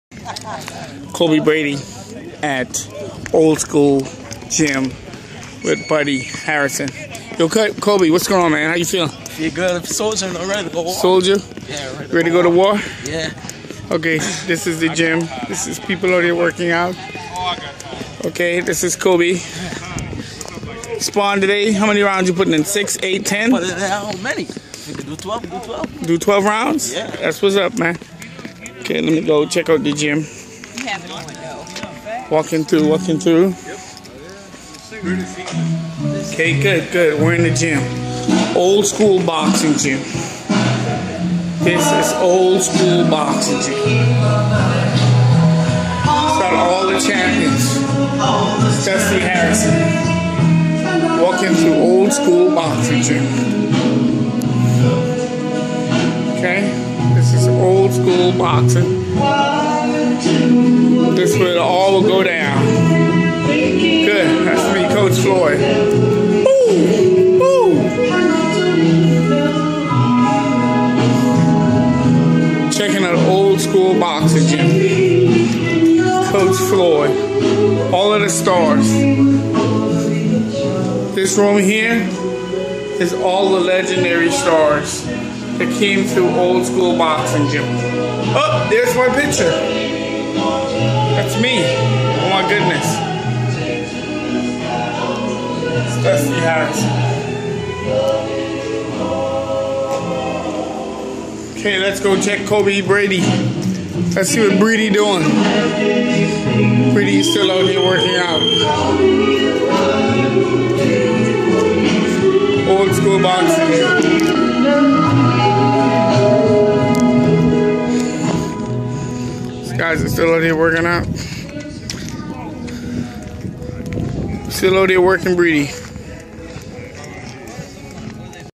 Coach Floyd and Cobia Breedy at old School boxing gym working put